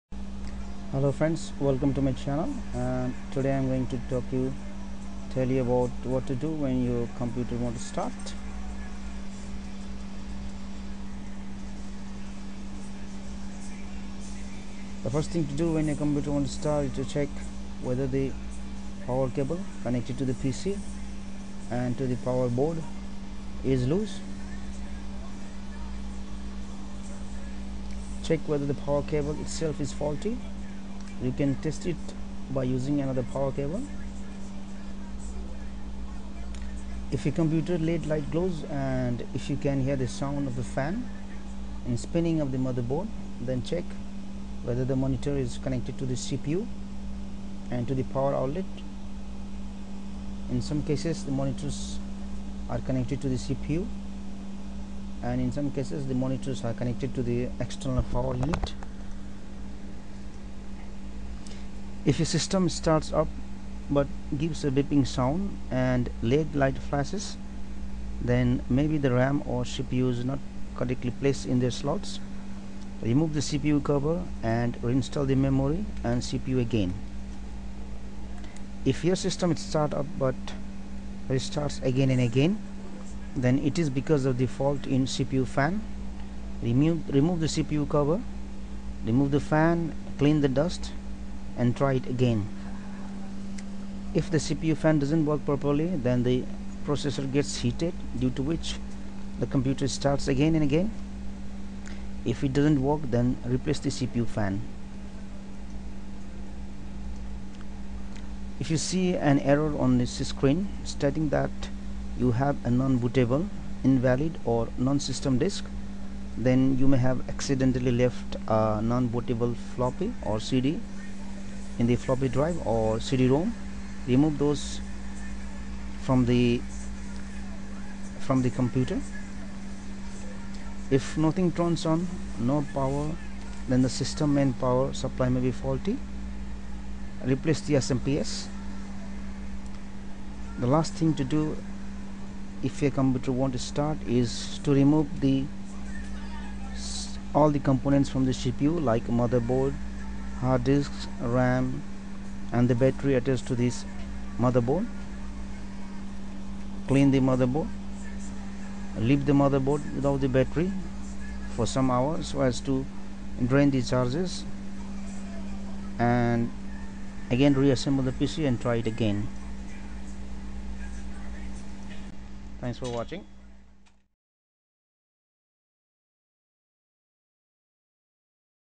Windows Troubleshooting Tutorial What to do when your computer won't start
Troubleshooting steps when your computer won't start. Follow the steps and restart the computer.